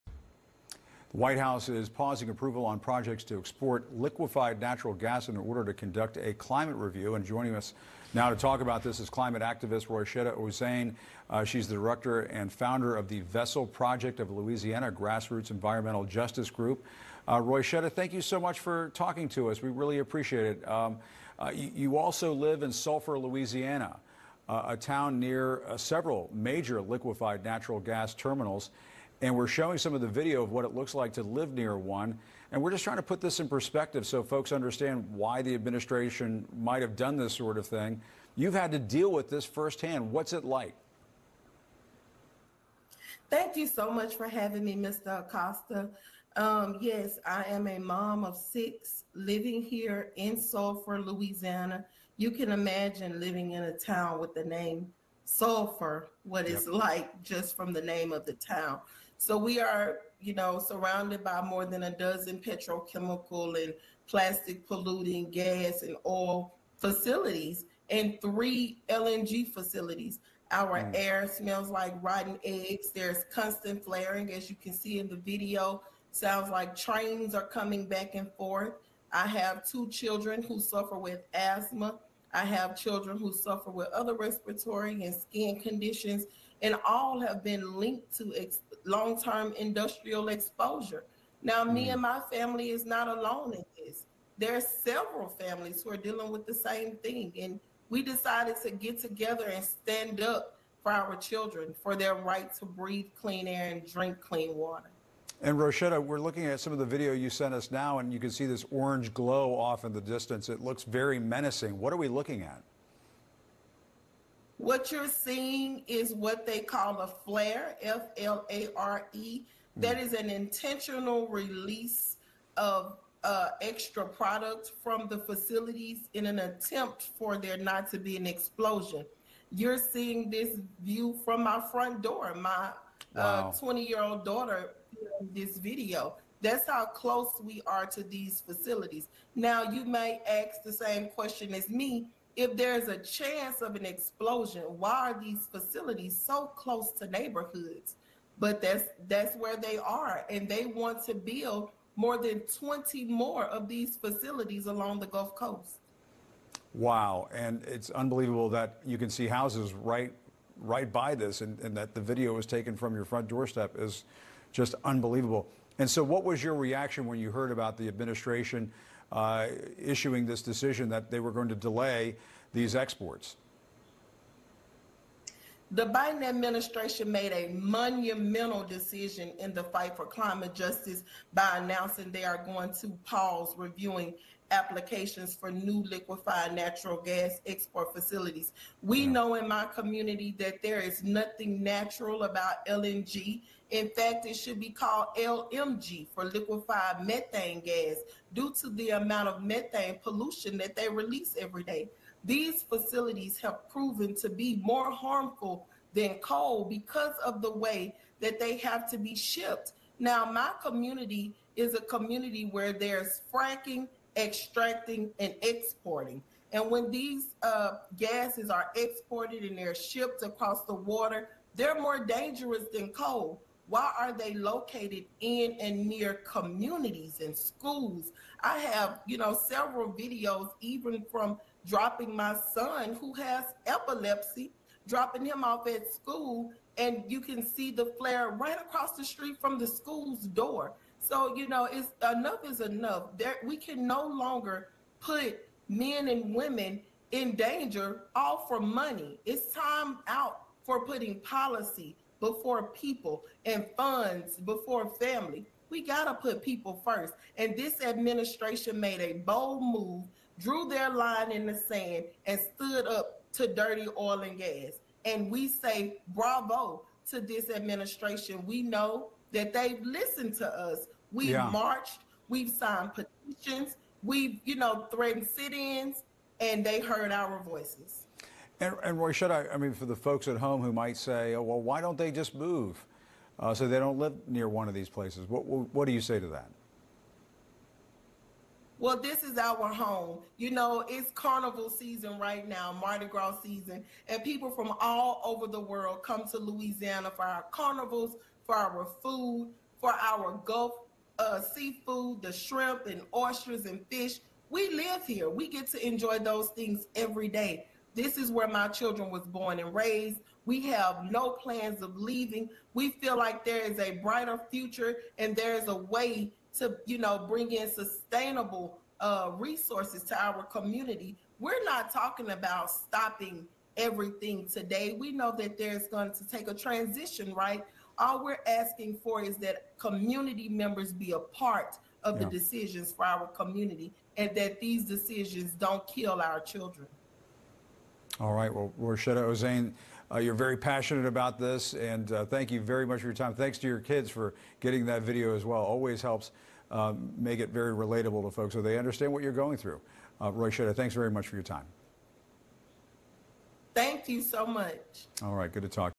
Roishetta Ozane on CNN to Respond to Recent LNG Pause
WATCH: Roishetta Ozane joins @CNN to ground in the recent LNG pause and what this means for her community ✊🏾

This is a major win that's a direct result of people power and frontline leadership across Texas, Louisiana and the Gulf South!